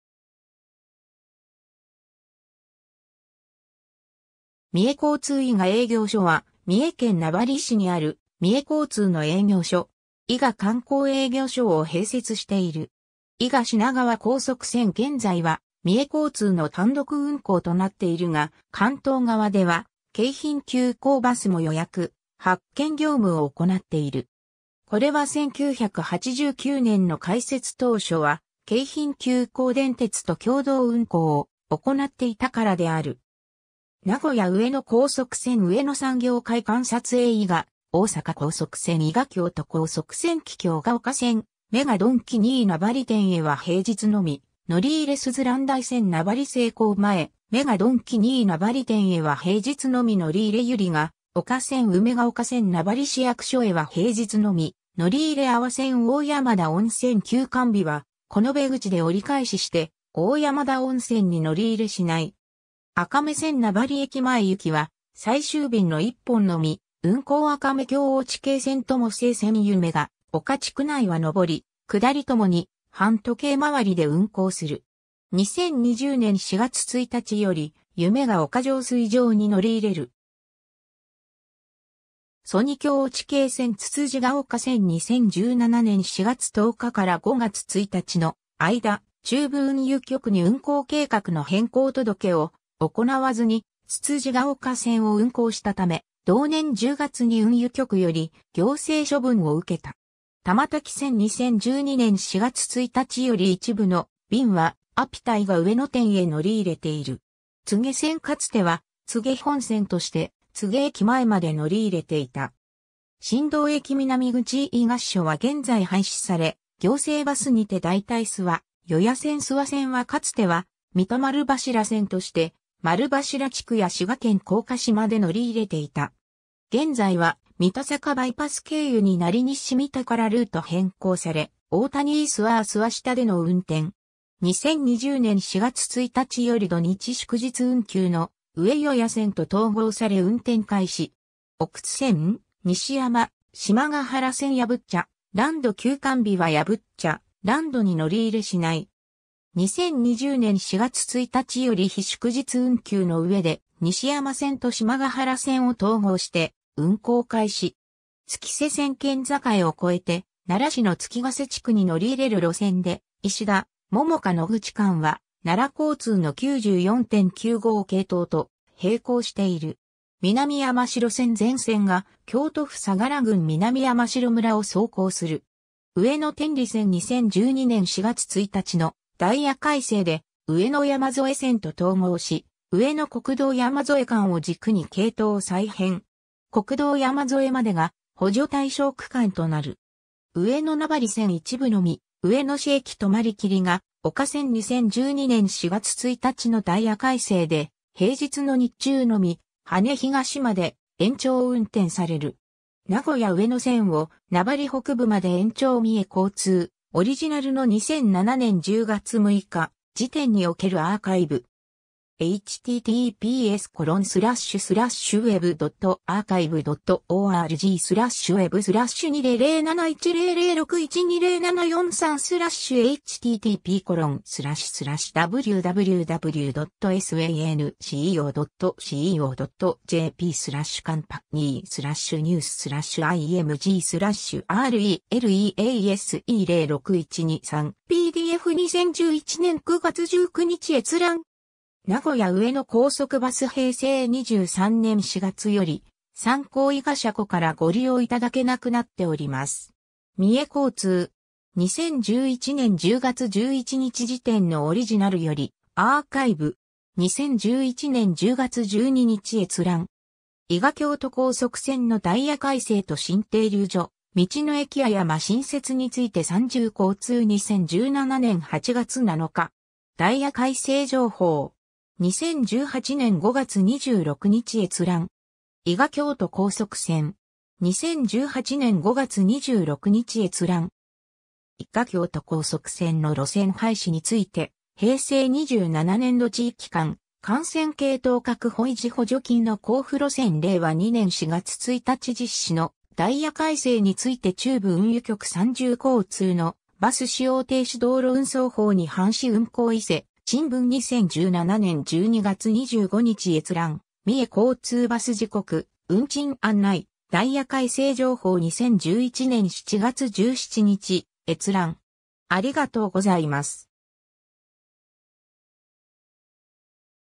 三重交通伊賀営業所
三重交通伊賀営業所（みえこうつういがえいぎょうしょ）は、三重県名張市にある、三重交通の営業所。
伊賀観光営業所を併設している。
 伊賀品川高速線（いが号） 現在は三重交通の単独運行となっているが、関東側では京浜急行バスも予約・発券業務を行っている。
これは1989年の開設当初は京浜急行電鉄（2003年10月1日に自動車事業を分社化）と共同運行を行っていたからである。
 名古屋上野高速線 上野産業会館撮影 伊賀大阪高速線（忍者ライナー） 伊賀京都高速線 桔梗が丘線 ※MEGAドンキUNY名張店へは平日のみ乗り入れ すずらん台線 ※名張西高前、MEGAドンキUNY名張店へは平日のみ乗り入れ 百合が丘線 梅が丘線 ※名張市役所へは平日のみ乗り入れ ◎阿波線 ※大山田温泉休館日は子延口で折返しして大山田温泉に乗り入れしない。
 赤目線 ※名張駅前ゆきは最終便の1本のみ運行 赤目香落渓線 友生線 ※ゆめが丘地区内は上り・下り共に反時計周り（ゆめドーム前→ゆめが丘四→ゆめが丘地区市民センター前→友生小学校前）で運行する。
2020年4月1日よりゆめが丘浄水場に乗り入れる。
 ◎曽爾香落渓線 つつじが丘線 ※2017年4月10日-5月1日の間、中部運輸局に運行計画の変更届を行わずにつつじヶ丘線を運行したため、同年10月に運輸局より行政処分を受けた。
 ◎玉滝線 ※2012年4月1日より一部の便はアピタ伊賀上野店へ乗り入れている。
 柘植線 ※かつては柘植本線として柘植駅前まで乗り入れていた。
 ※新堂駅南口ー伊賀支所は現在廃止され行政バスにて代替 諏訪・予野線 ※諏訪線はかつては三田丸柱線として、丸柱地区や滋賀県甲賀市（旧信楽町）まで乗り入れていた。
現在は三田坂バイパス経由になり西三田からルート変更され大谷ー諏訪ー諏訪下出の運転。
2020年4月1日より土日祝日運休の上予野線と統合され運転開始。
 奥津線 西山・島ヶ原線 ※やぶっちゃランド休館日はやぶっちゃランドに乗り入れしない。
2020年4月1日より日祝日運休の上で西山線と島ヶ原線を統合して運行開始。
 月瀬線 県境を超えて奈良市の月ヶ瀬地区に乗り入れる路線で、石打 - 桃香野口間は奈良交通の94・95系統（JR奈良駅 - 石打）と並行している（いずれも便数少ないが、奈良市中心部まで乗り継ぐことは可能）。
 南山城線 ※全線が京都府相楽郡南山城村を走行する。
 ◎上野天理線 ※2012年4月1日のダイヤ改正で、上野山添線（上野産業会館 - 菅生間）と統合し、上野 - 国道山添間を軸に系統を再編。
 ※国道山添までが補助対象区間となる。
 ◎上野名張線 ※一部のみ上野市駅止まり 桐が丘線 ※2012年4月1日のダイヤ改正で、平日の日中のみ、羽根東（青山Aコープ前）まで延長運転される。
 ^ “「名古屋上野線」を名張北部まで延長” (PDF) (プレスリリース), 三重交通, (2006年12月8日), オリジナルの2007年10月6日時点におけるアーカイブ。
2011年9月19日閲覧。
 ^ “名古屋上野高速バス　 平成23年4月より三交伊賀車庫（名張駅西田原）からご利用いただけなくなっております。”. 三重交通. 2011年10月11日時点のオリジナルよりアーカイブ。
2011年10月12日閲覧。
 ^ 「伊賀京都高速線」のダイヤ改正と新停留所「道の駅あやま」新設について　三重交通　2017年8月7日。
 ^ “ダイヤ改正情報”. 2018年5月26日閲覧。
 ^ “伊賀京都高速線”. 2018年5月26日閲覧。
 ^ 伊賀京都高速線の路線廃止について(三重交通 2018年8月23日) ^ 「平成27年度地域間幹線系統確保維持補助金の交付路線」 (PDF) ^ 令和2年4月1日実施のダイヤ改正について ^ 中部運輸局　三重交通のバス使用停止　道路運送法に反し運行 伊勢新聞（2017年10月20日）2017年12月25日閲覧 ^ 三重交通バス時刻・運賃案内「ダイヤ改正情報」2011年7月17日閲覧